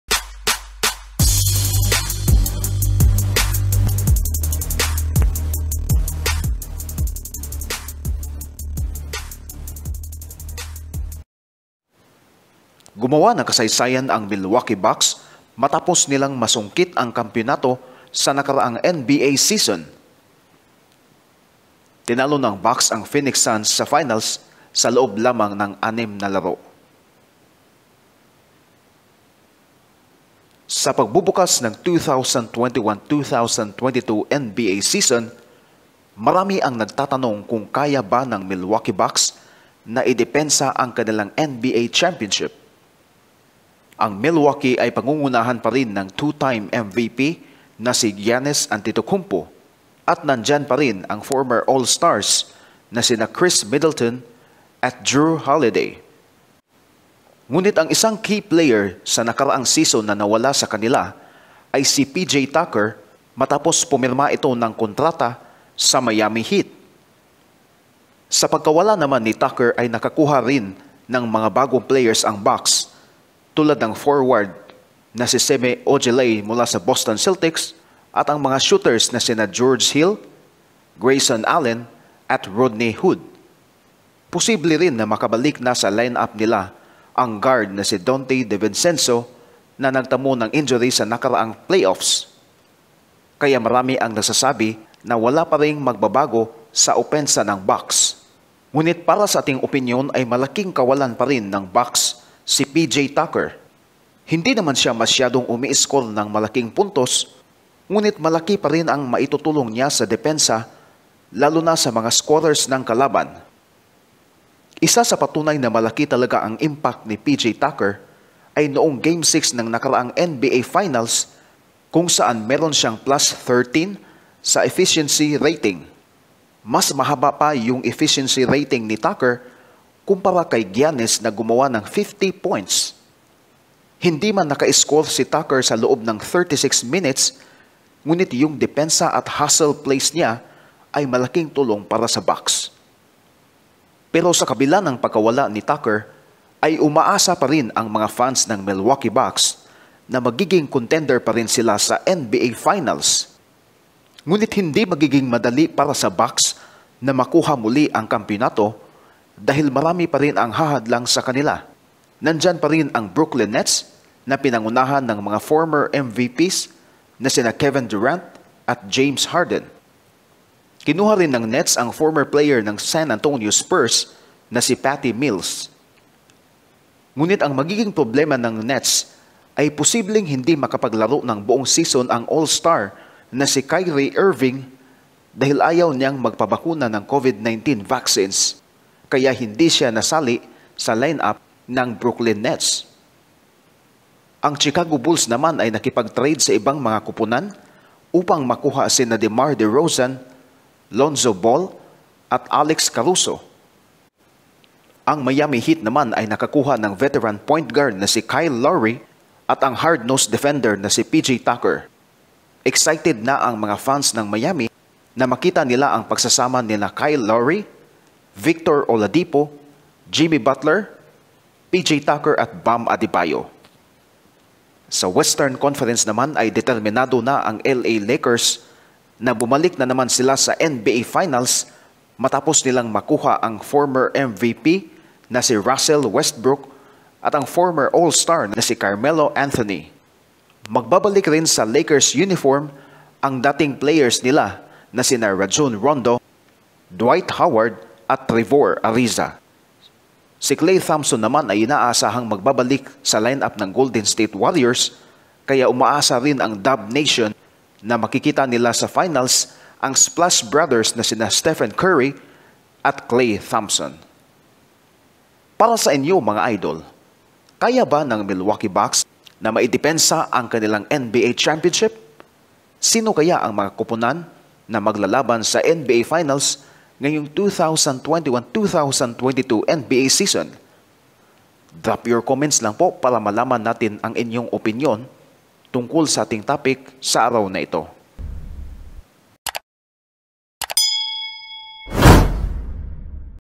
Madepensahan kaya ng Milwaukee Bucks ang NBA Title? | LA Lakers at Brooklyn Nets target ang Finals
Sa pagsisimula ng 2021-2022 NBA Regular Season, ano ang inyong early prediction? Kaya ba ng Milwaukee Bucks na madepensahan ang NBA championship?
Ang Superteams na Los Angeles Lakers at Brooklyn Nets ay gusto rin makuha ang kampeonato ngayong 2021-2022 NBA Season.

Ano sa tingin ninyo mga NBA fanatics?